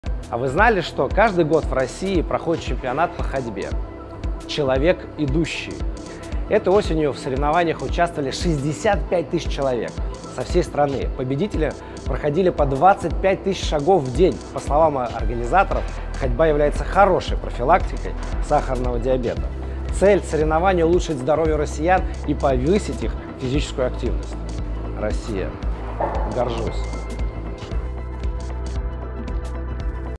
Чемпионат «Человек идущий»: профилактика и здоровье
Дмитрий Носов, дзюдоист и политик, о чемпионате «Человек идущий» 🚶‍♂️

А вы знали, что в России каждый год проходит чемпионат по ходьбе «Человек идущий»? Осенью 2022 в соревнованиях участвовали 65 000 человек со всей страны.

Победители проходили по 25 000 шагов в день. По словам организаторов, ходьба является хорошей профилактикой сахарного диабета. Цель соревнования – улучшить здоровье россиян и повысить их физическую активность.

Россия, горжусь! 🇷🇺